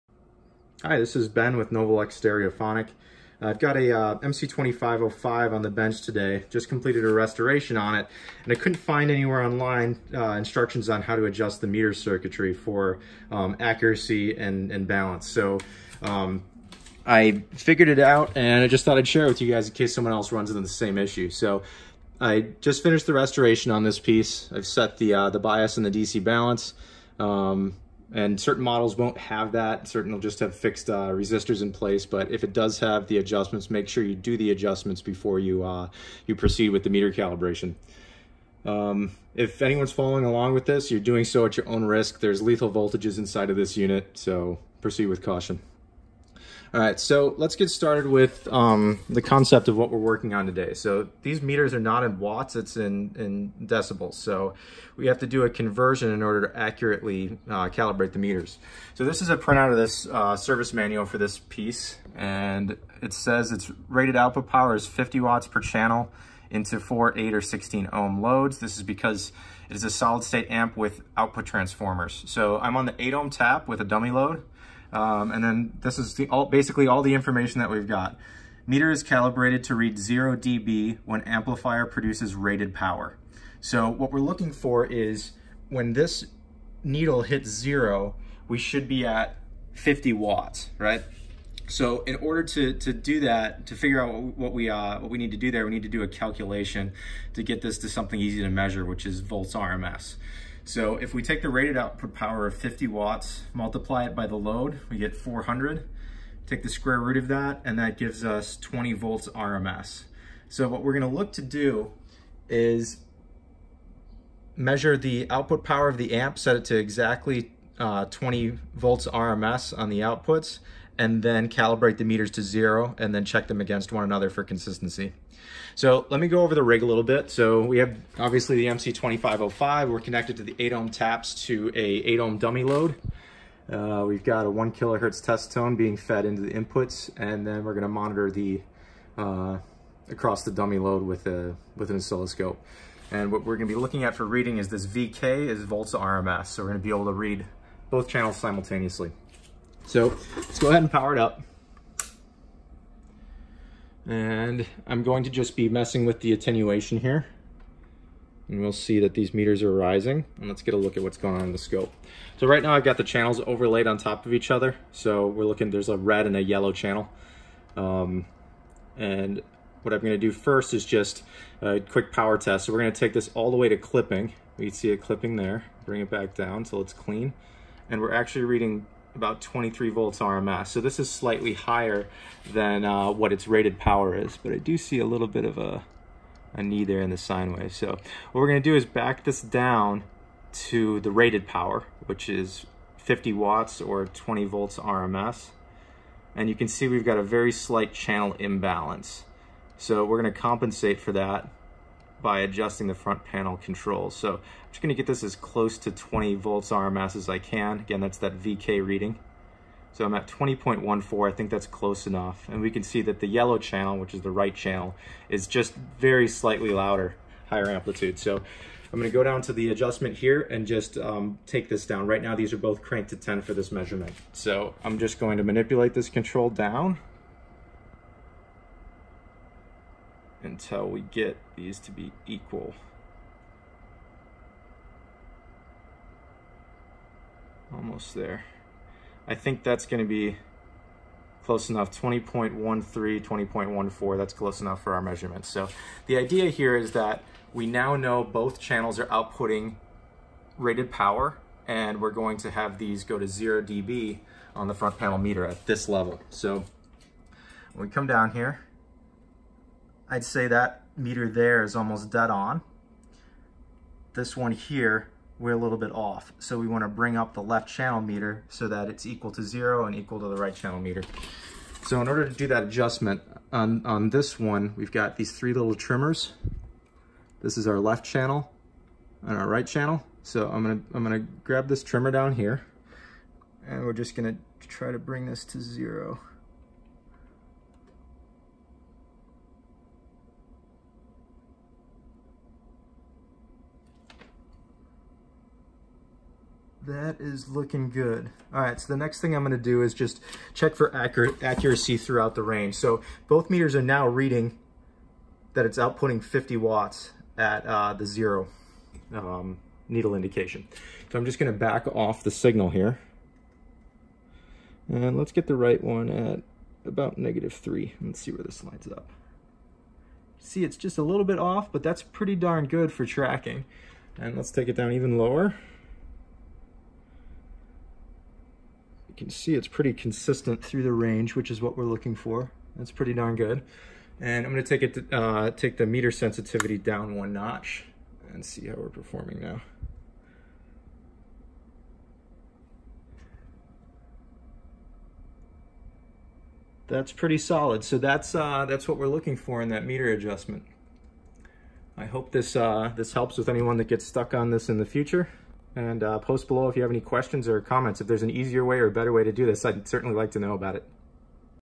McIntosh MC2505 Meter Adjustment / Calibration
DANGER - High Voltage Present! Follow along at your own risk.

This video demonstrates how to measure the rated output power on the McIntosh MC2505 and how to use that measurement to calibrate the front panel meters.

NOTE: I shot this video with the unit on its side as I had just finished the amplifier DC Balance and Bias adjustments. For best results, make the meter adjustment while the amp is sitting in the normal position.